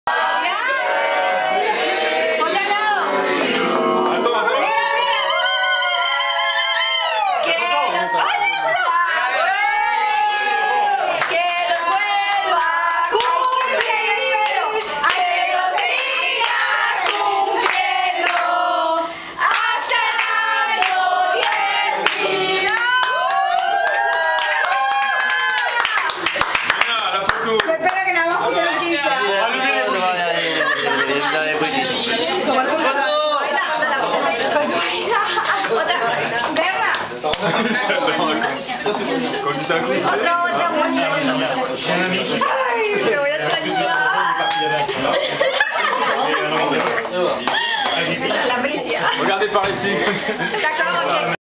Gina 30 bis Birthday
30 July 2011 at Patrick's house celebrating Gina's birthday